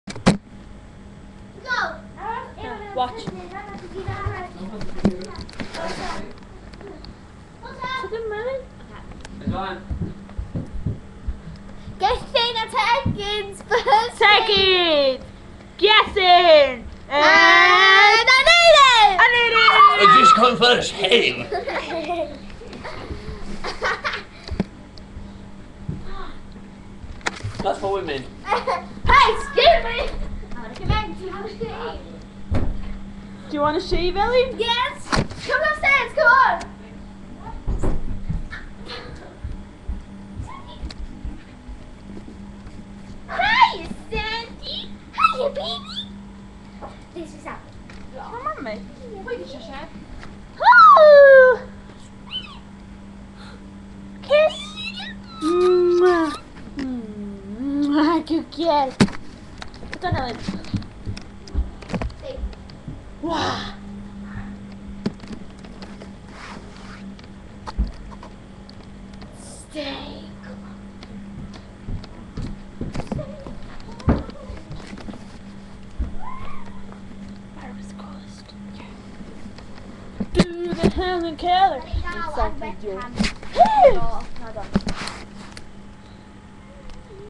Gethin and Tegids first shave (Including Aneurin.)
Geth and Tegs' first shavee !!!
Geth - 11
Tegid - 9
Featuring''''
the dog - Sandy
The mum - Meinir
Elin and Carina - Elin and Carina
and Aneurin - Aneurin (crazy old dude ) xx